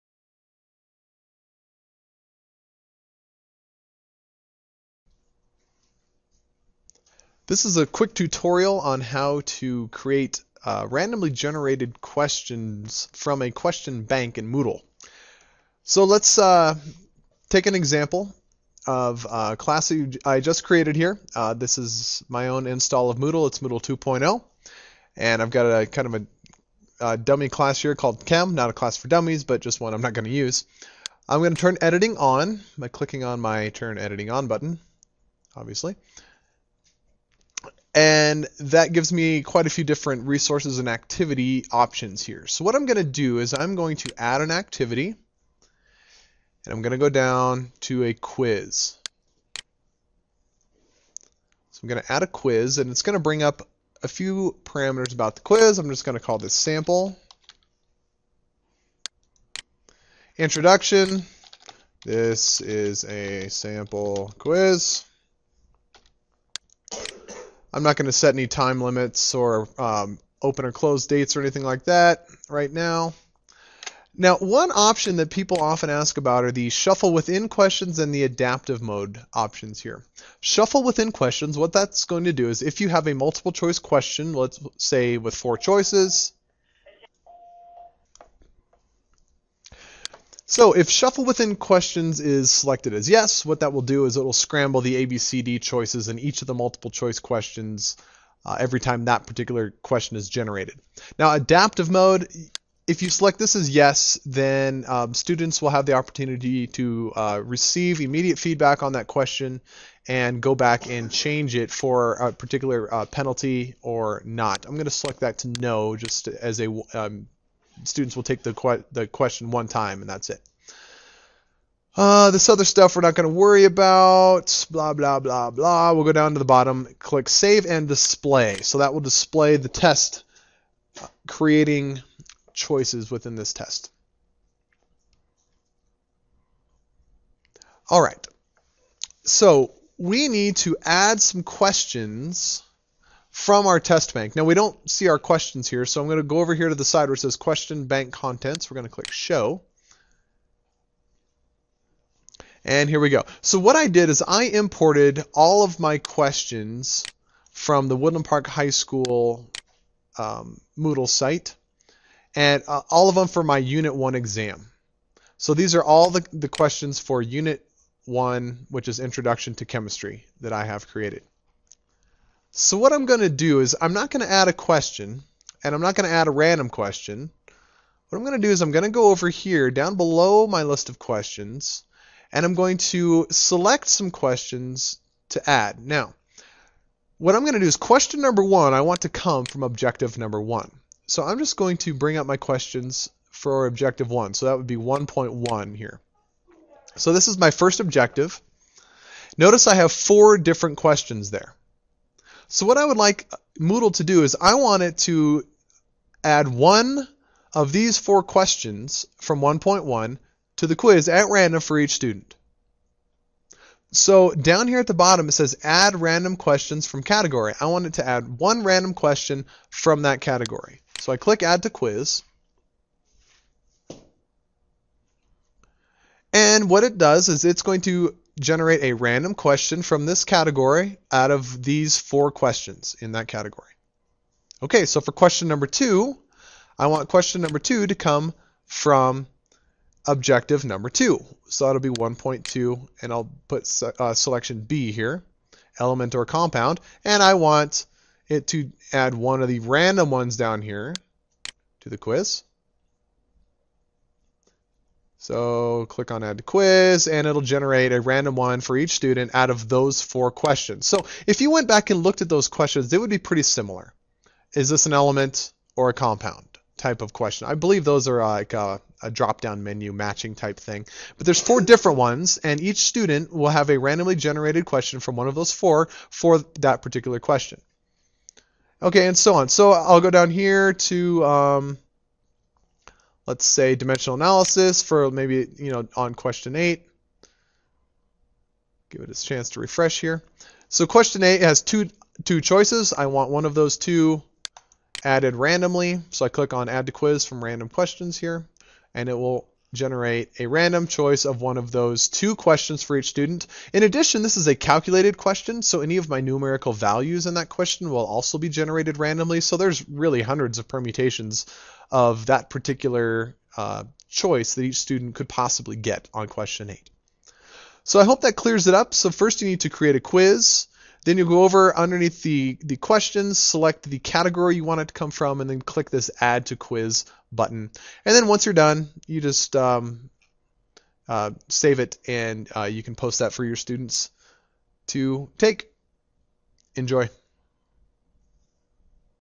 Generating Random Questions in a Moodle Quiz
A tutorial on how to make Moodle generate a random question from a test bank in a Quiz.